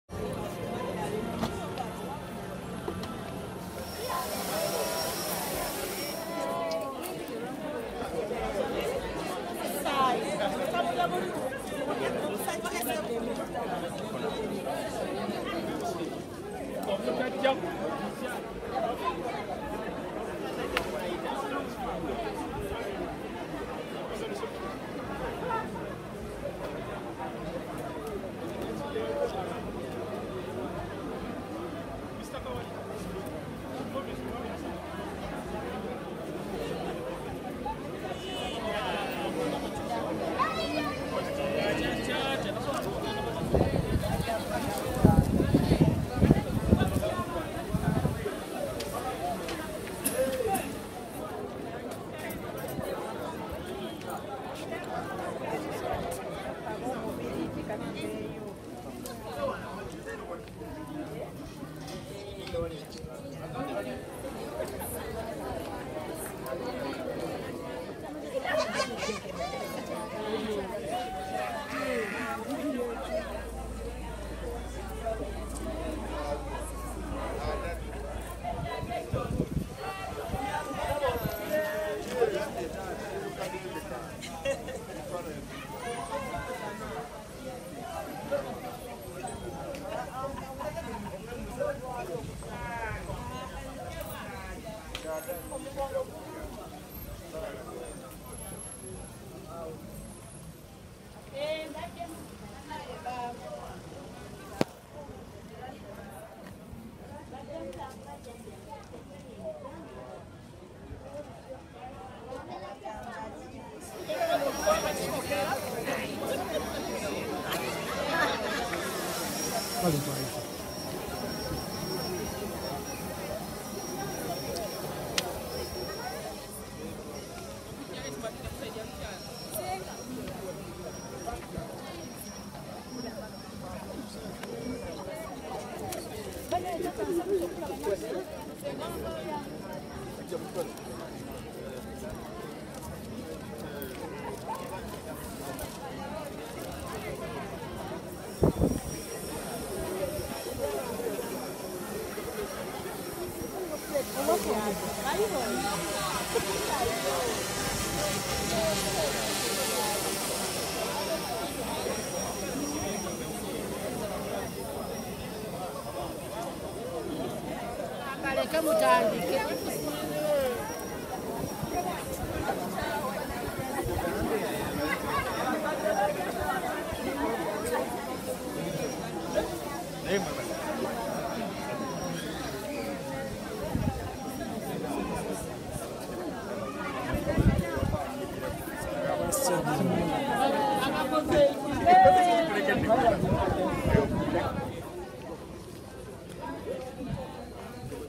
How to Curtsy In Uganda | Greeting In Uganda | Showing respect with postures.
In Uganda especially in the central region women or young girls kneel down or Curtsy while greeting people especially if those people are elders or their superiors. It is a sign of showing respect to people. Some men kneel especially while greeting the Kabaka in Buganda.

In the video people are greeting each other at a wedding church ceremony. It is a place where you come to meet people you take long to associate with. Other parts of the world, British Prime Minister curtsies to the queen and other senior royals. You can also hug, shake hands or bow is also a sign for f respect. Some countries in Africa also Curtsy to the elders.